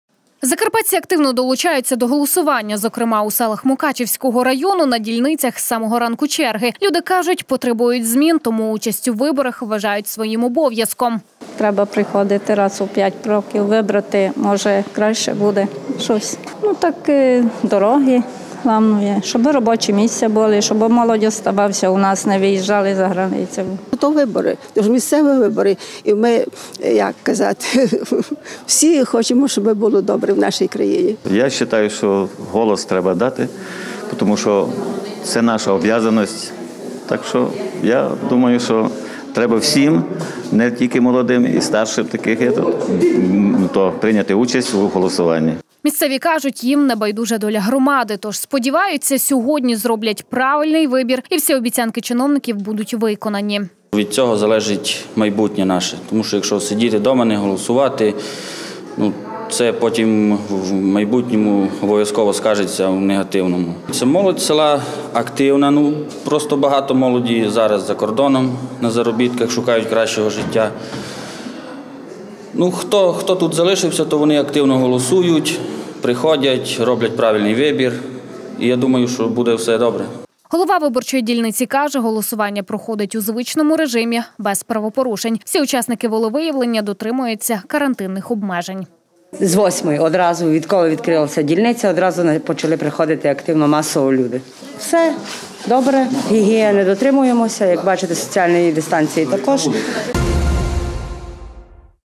Місцеві вибори
Закарпатці активно долучаються до голосування. Зокрема, у селах Мукачівської ОТГ на дільницях  з самого ранку черги. Люди кажуть, потребують змін, тому участь у виборах вважають своїм обов’язком.
Місцеві кажуть, їм не байдужа доля громади. Тож сподіваються, сьогодні зроблять правильний вибір і всі обіцянки чиновників будуть виконані. Голова виборчої дільниці каже, голосування проходить у звичному режимі. Без правопорушень. Всі учасники волевиявлення дотримуються карантинних обмежень.